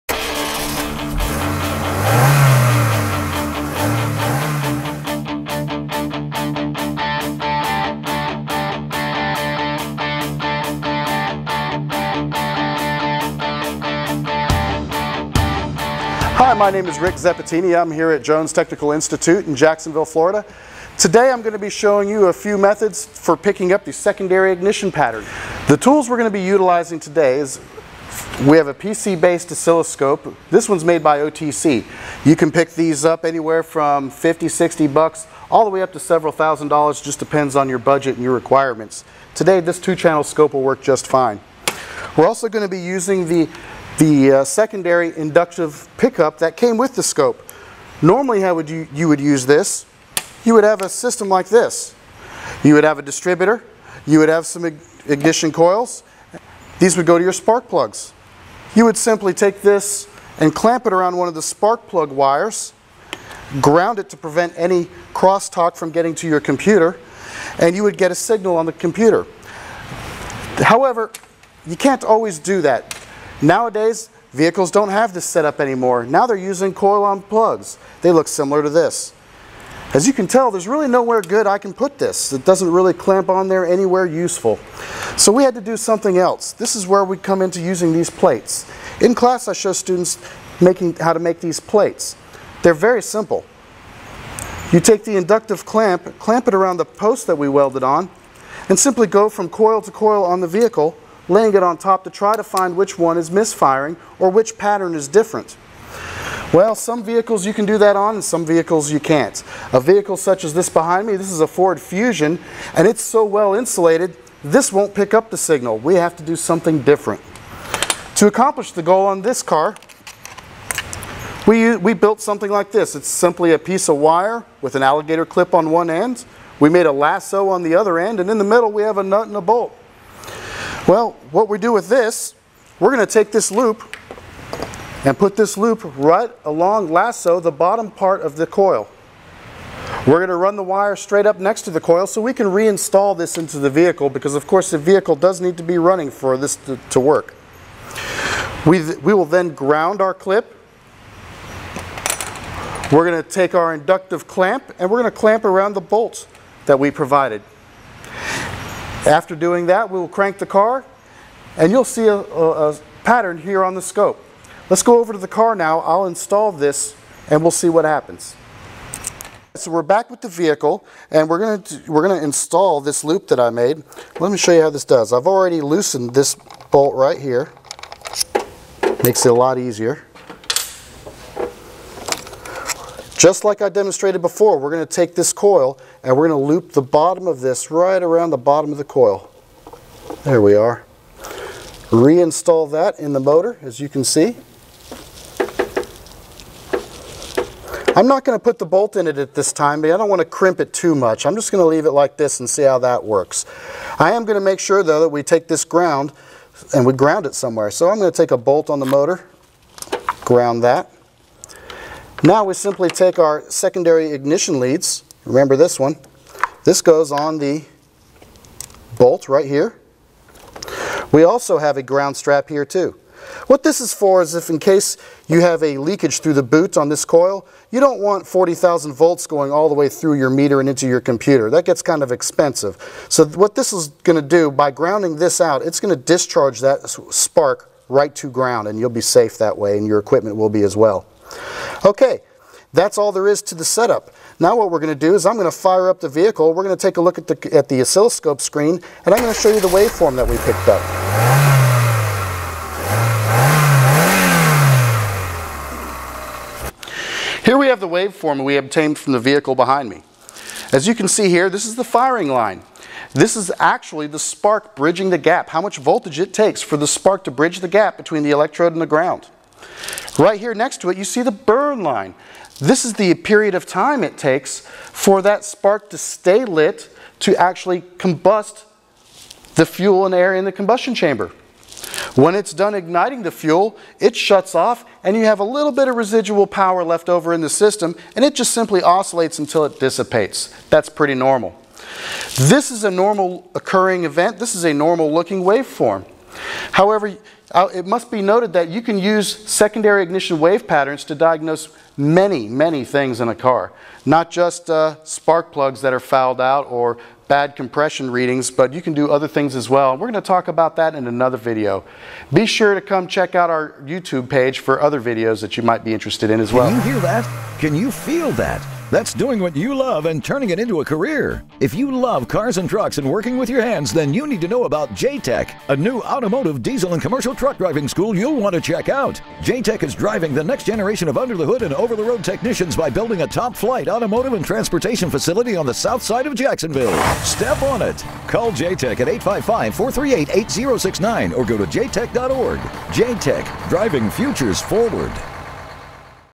Picking up the secondary ignition pattern using an oscilloscope
This video was created to show how to pick up the secondary ignition pattern using a simple homemade tool and an oscilloscope.